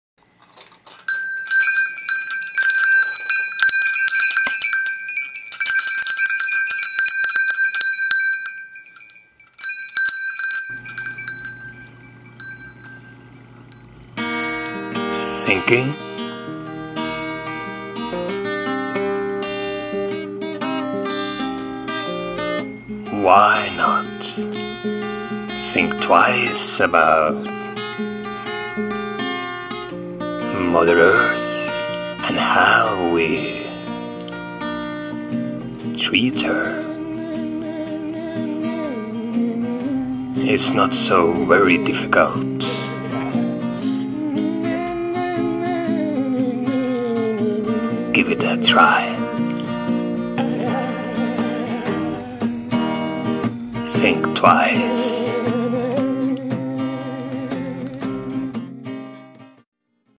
Think twice
Cinepoetry: All of us live on this planet in this enormous universe - why not take care of this "Mother" - ? - she feeds us.